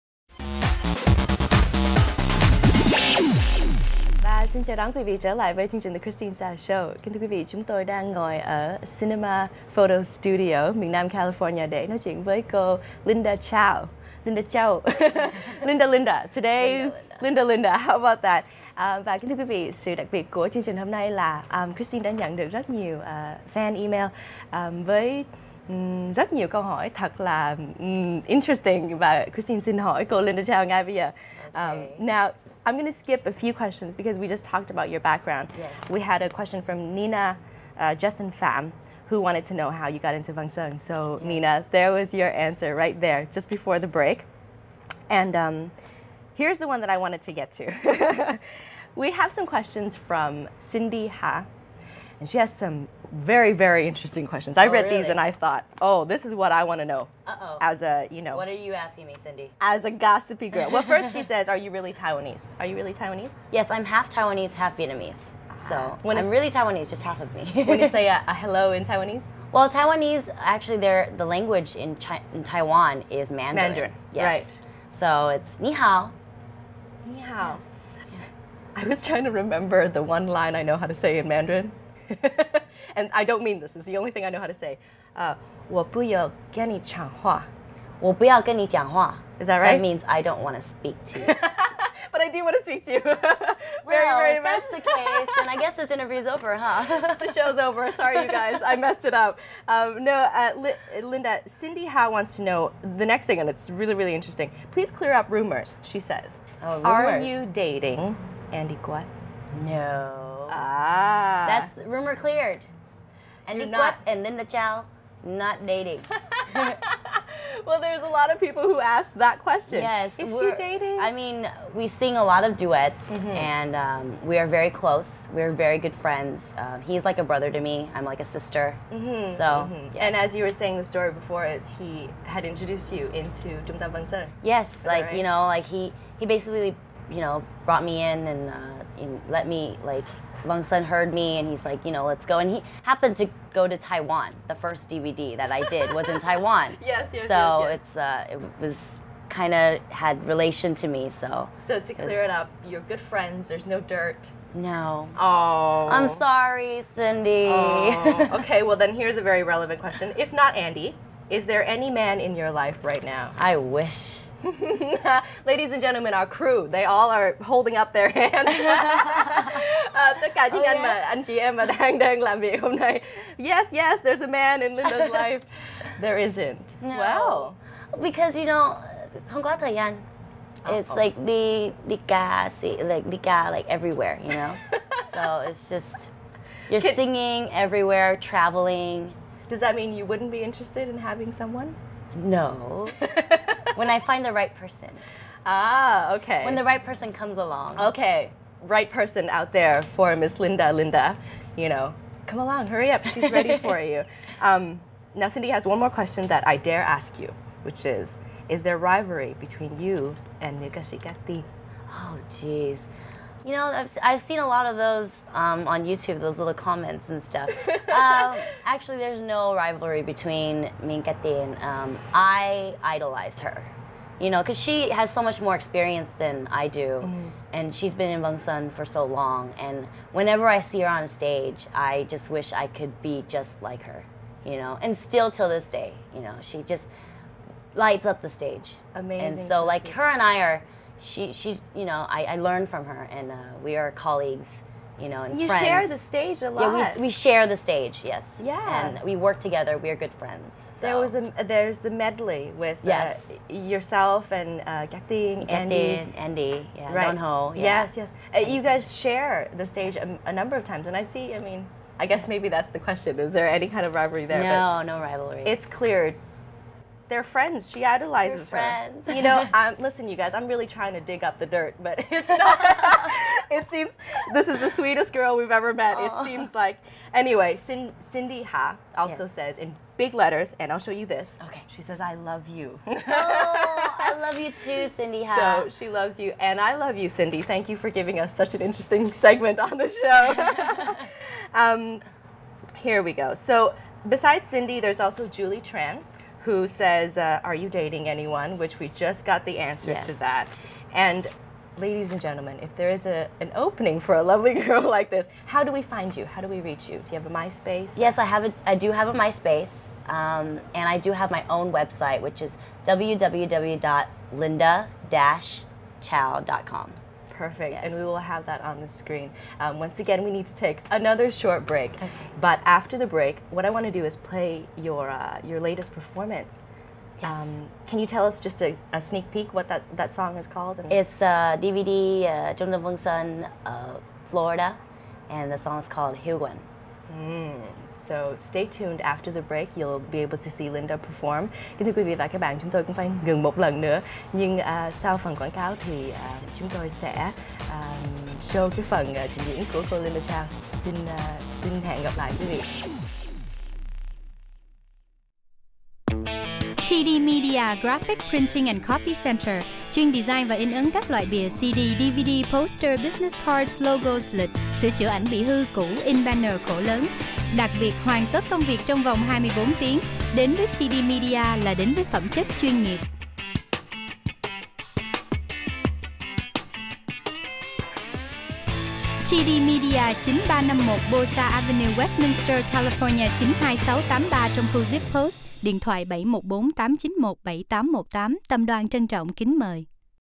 Kristine Sa & Linda Chou 2009 [P2 of 3]
Multilingual popstar Linda Chou sits down with Kristine Sa at Cinema Photo Studio to answer some very hard questions from her fans.